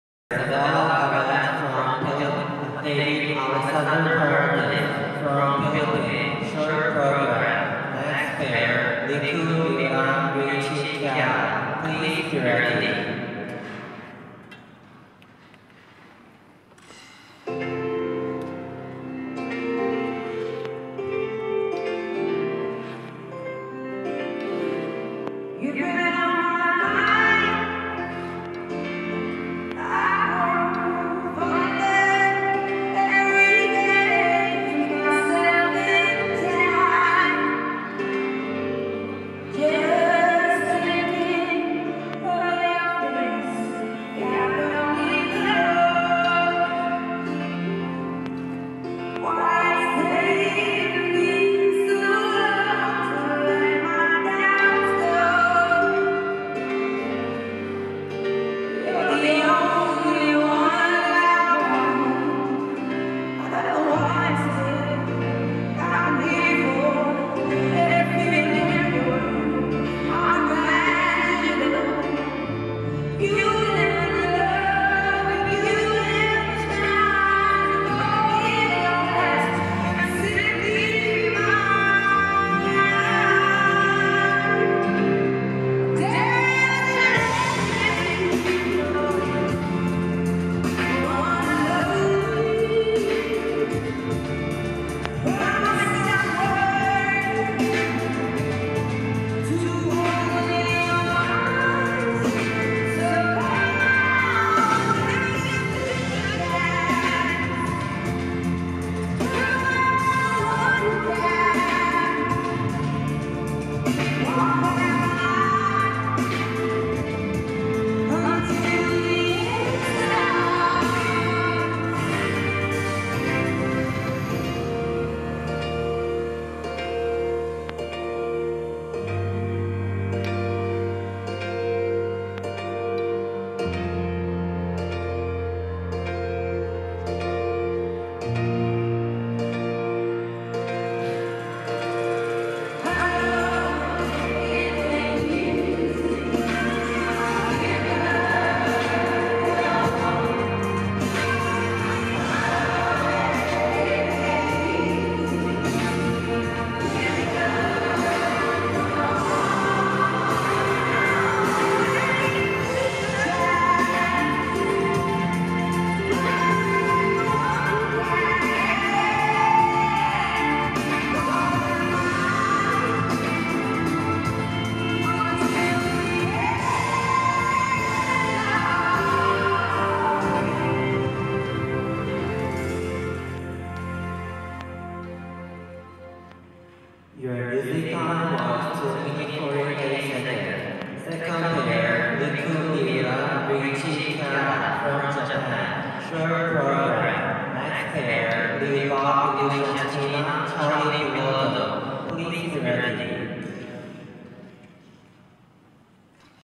Isabella GAMEZ & David-Alexandre PARADIS (PHI) Pairs - SP Rehearsal 1-1ㅣ2020-02-06 4CC - Day3.
이사벨라 가메즈 & 데이빗-알렉산드레 파라디스 |
ISU Four Continents Figure Skating Championships 2020
Feb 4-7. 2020. Mokdong Indoor Ice Rink, Seoul Republic of KOREA (South)ㅣ
Day3 | Feb 6. Dress Rehearsal, Competition Warmup

Map.

2020년 ISU 사대륙 피겨스케이팅 선수권대회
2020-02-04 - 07. | 대한민국 서울 목동 와이키키 실내빙상장(목동아이스링크) |
대회 3일차 (경기 2일차) 드레스 리허설, 경기 웜업